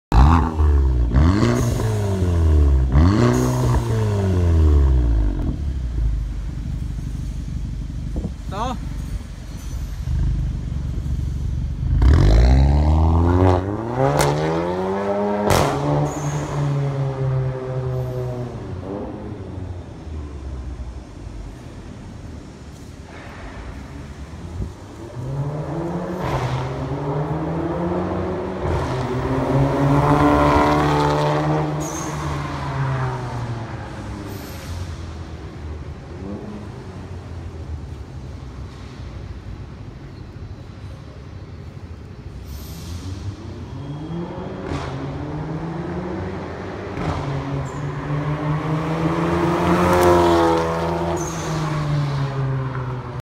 Audi A3 2.0T RES Racing Titanium Custom Exhaust Tips Auto Parts
Audi A3 updates RES Racing valvtronic catback system which is made of Titanium , you even can control the exhaust with "RES" APP via smart phones .
You can open variable exhaust valves when you play, close it when in urban area.
RES exhaust is very convenient to play .